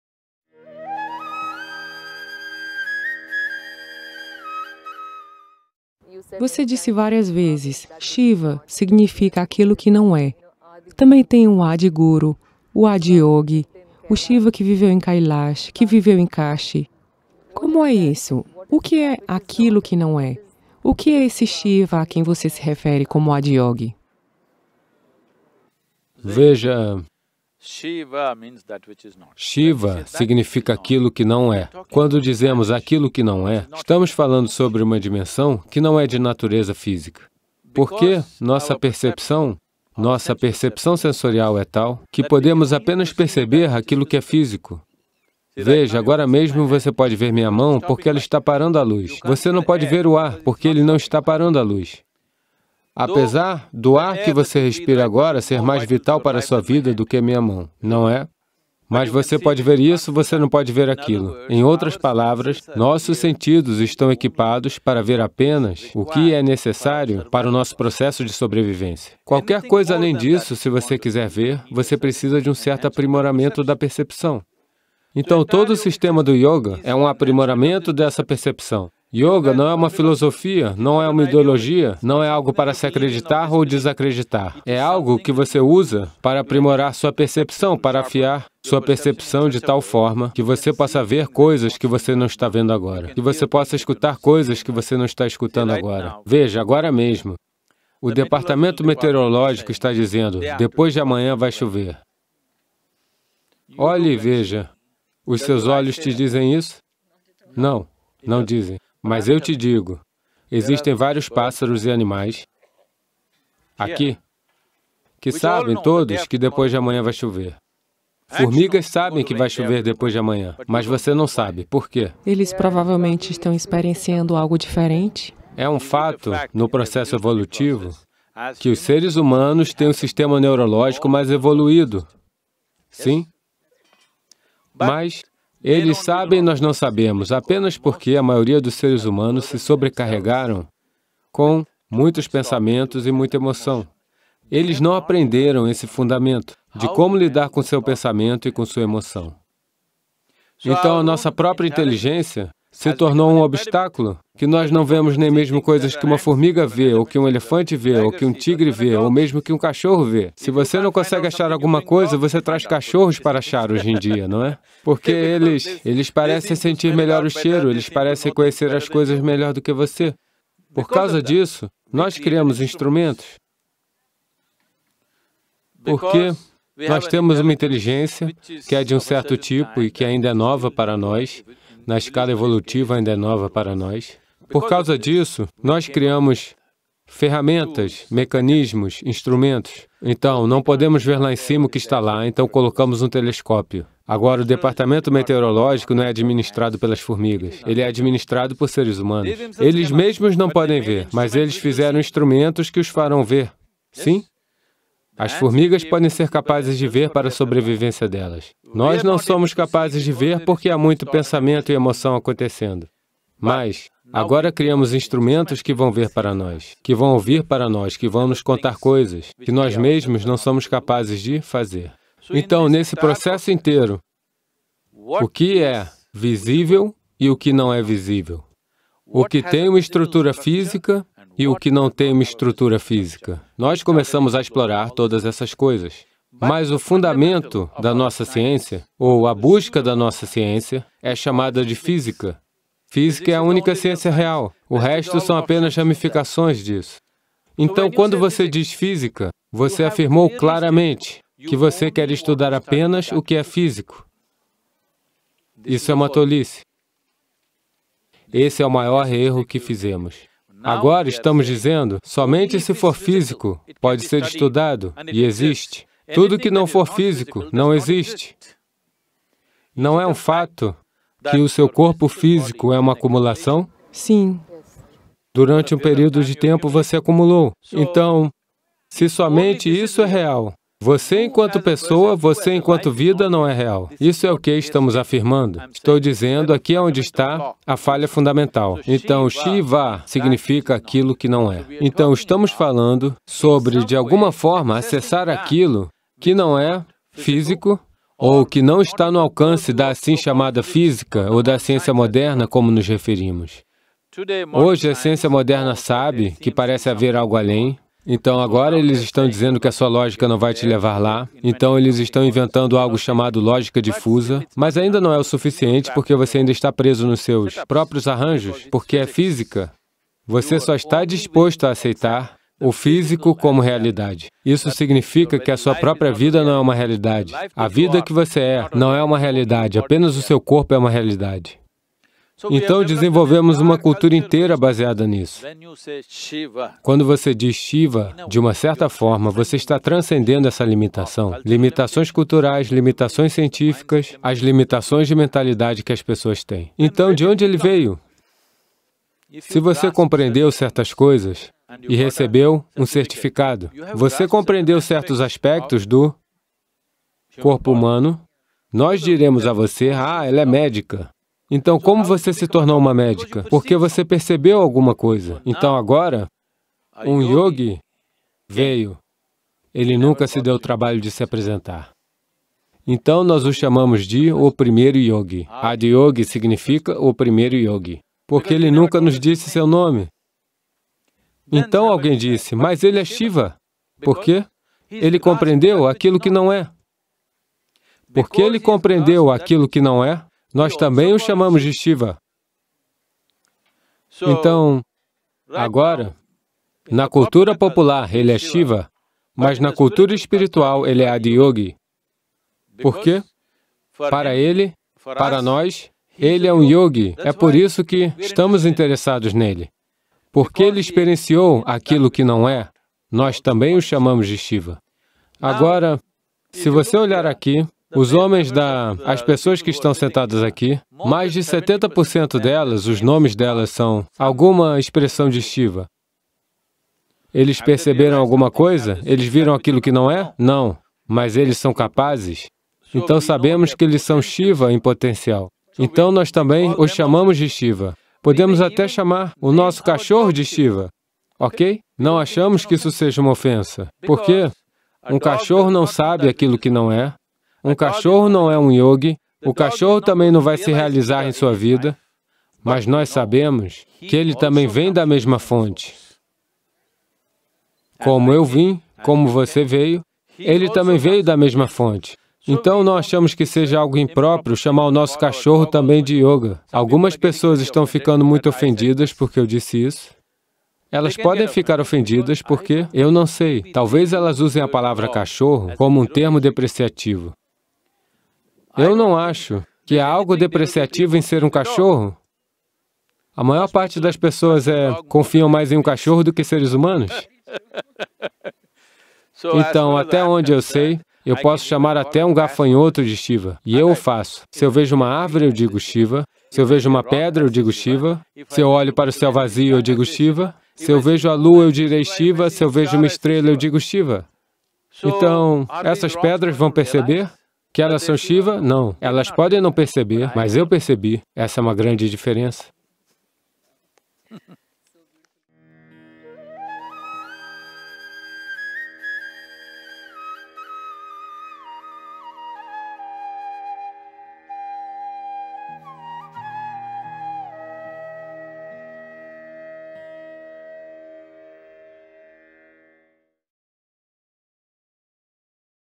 Porque Shiva É Significativo Ainda Hoje? | Sadhguru Português
Shiva, Adi Guru, Adiyogi - esses nomes descrevem um indivíduo real ou algo mais? A palavra "Shiva", explica Sadhguru, significa "aquilo que não é" - uma dimensão que não é de natureza física. Nossos sentidos são voltados para detectar apenas o físico. No entanto, o yoga é uma ferramenta que se pode usar para melhorar a percepção além do físico.

Sadhguru Palestra @ TV5 Interview, Fevereiro/2017, Estatua de Adiyogi de 112 pés de altura, no Isha Yoga Center, Coimbatore.